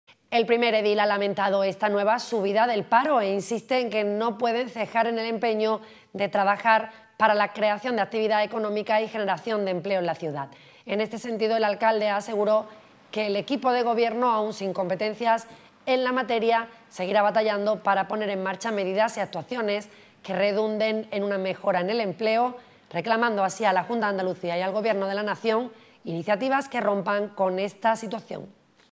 Landaluce pide medidas a las administraciones para la creación de empleo
El primer edil ha valorado negativamente esta nueva subida del paro, insistiendo en que no pueden cejar en el empeño de trabajar para la creación de actividad económica y generación de empleo en nuestra ciudad. En este sentido, el primer edil aseguró que este equipo de gobierno, aun sin competencias en esta materia, seguirá batallando para poner en marcha medidas y actuaciones que redundan en una mejora en el empleo, reclamando así a la Junta de Andalucía y al Gobierno de la Nación iniciativas que rompan con esta situación.